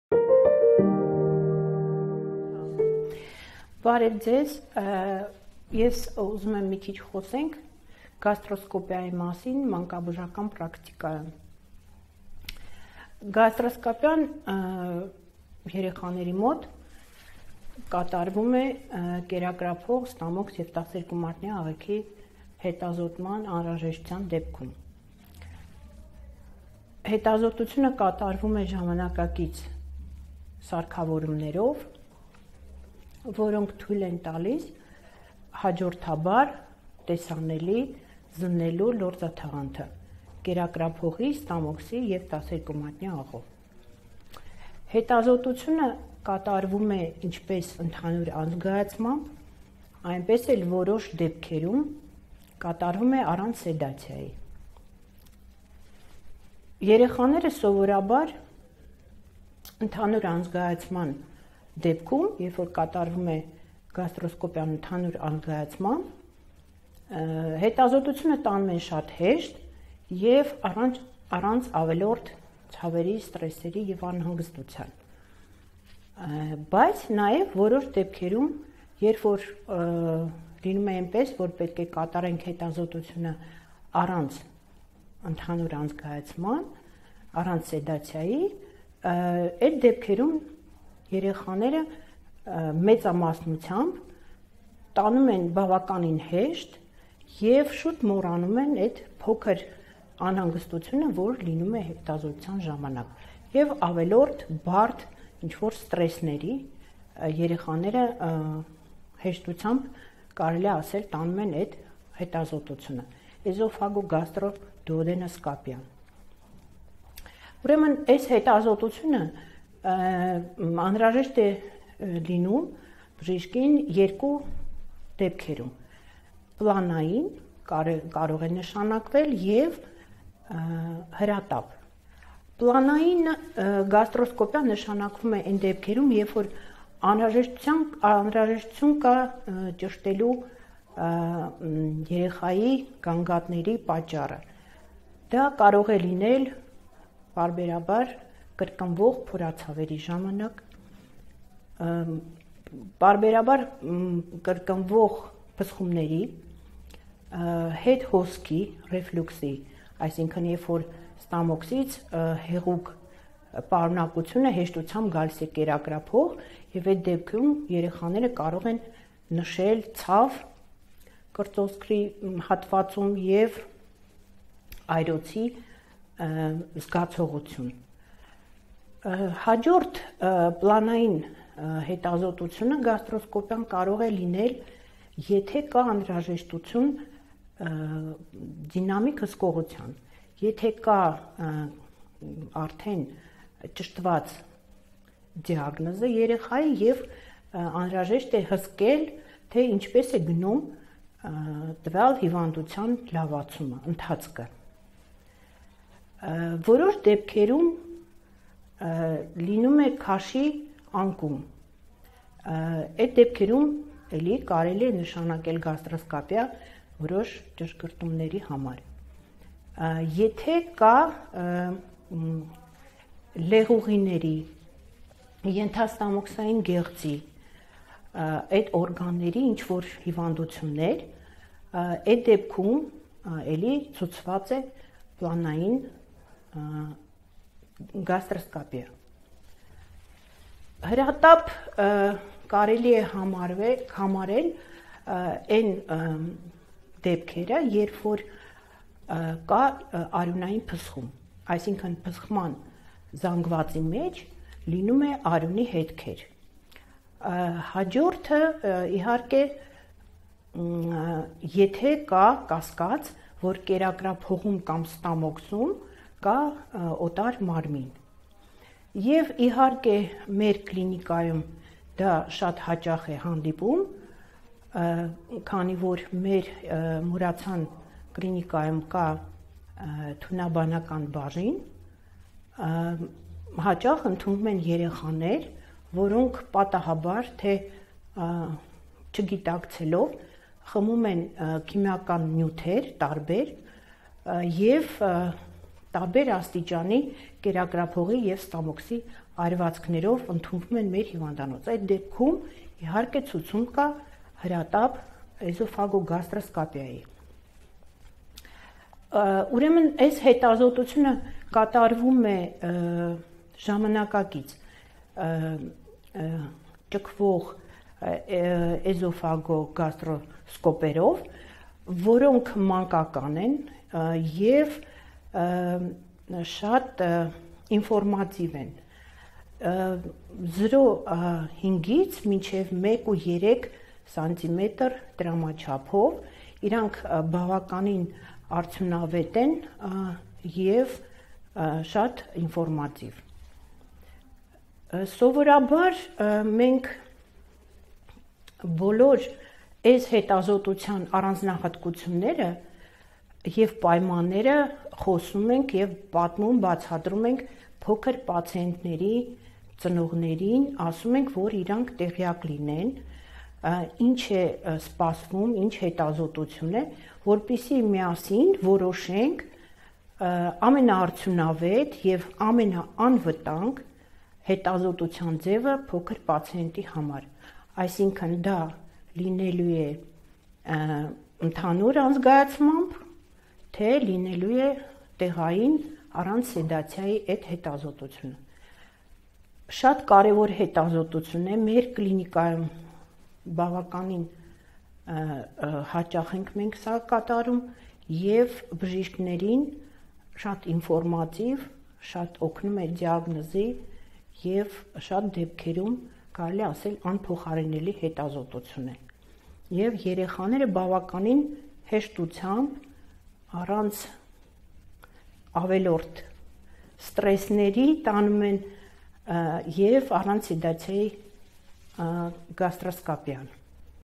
Գաստրոսկոպիան «Մուրացան» համալսարանական հիվանդանոցում կատարվում է ժամանակակից սարքավորումներով
👉Գաստրոսկոպիան կերակրափողի, ստամոքսի և 12-մատնյա աղու էնդոսկոպիկ հետազոտությունն է, որը «Մուրացան» համալսարանական հիվանդանոցային համալիրում կատարվում է ժամանակակից սարքավորումներով:
✅Ախտորոշիչ կենտրոնի էնդոսկոպիայի բժիշկ Նաիրա Վանյանը խոսել է երեխաների մոտ գաստրոսկոպիկ հետազոտություն կատարելու մասին:
Ինչպես մեծահասակներին, այնպես էլ երեխաներին գաստրոսկոպիա կարող է կատարվել միայն բժշկի ցուցմամբ:
✅Գաստրոէնտերոլոգը փաստում է, որ երեխաները ընդհանուր անզգայացում կատարելու դեպքում սովորաբար հետազոտությունը հեշտ են տանում, առանց ավելորդ ցավի ուղեկցության և սթրեսի: